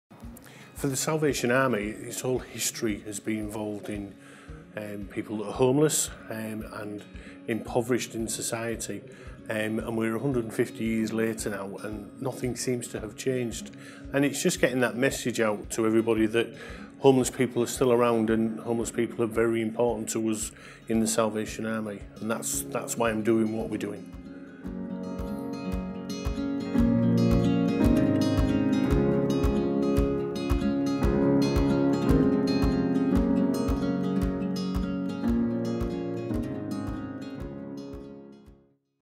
Sleep Easy 2019 - Interview with Patrick Tamminen #1
🎞| Here is Patrick Tamminen, Service Manager of Salvation Army Hull, talking about YMCA Humber's Sleep Easy 2019.

One night can make a lifetime of difference!
Next year we are hosting our tenth annual Sleep Easy Campaign.